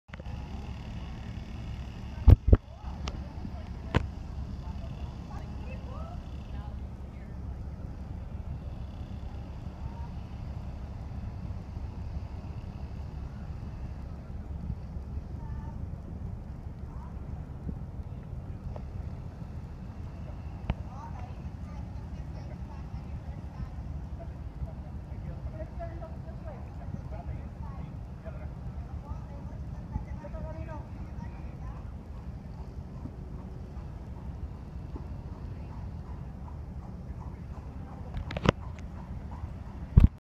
Key West
Key West Cruise Ship and Crystal Clear Waters!!!